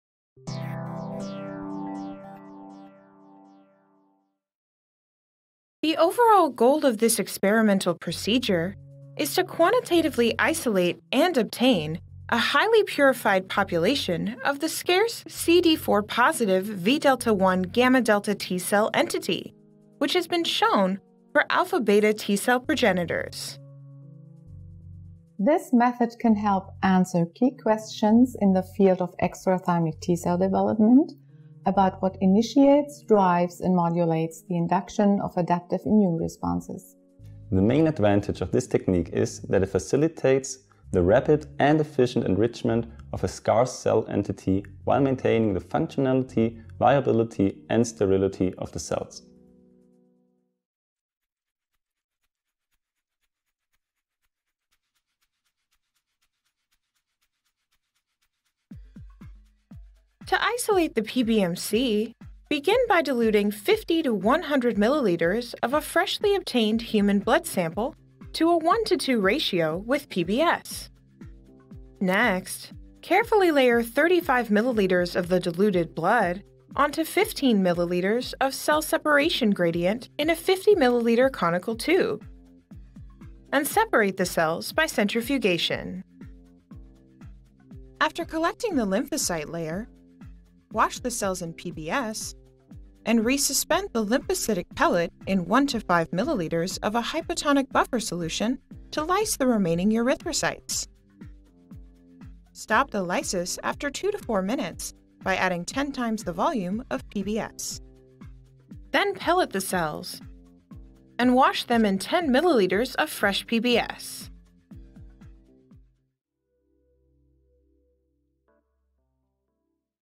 Isolation and Ex Vivo culture of T Cells | Protocol Preview
Isolation and Ex Vivo Culture of Vδ1+CD4+γδ T Cells, an Extrathymic αβT-cell Progenitor -  a 2 minute Preview of the Experimental Protocol

Christian Welker, Rupert Handgretinger, Karin Schilbach
Children&#39;s University Hospital Tübingen, Deptartment of Hematology and Oncology;

Here, we provide an optimized protocol for the isolation and cloning of the scarce T-cell entity of peripheral Vδ1+CD4+ T cells that is, as we showed recently, an extrathymic αβ T-cell progenitor. This technique allows to quantitatively isolate, clone and efficiently expand these cells in ex vivo culture.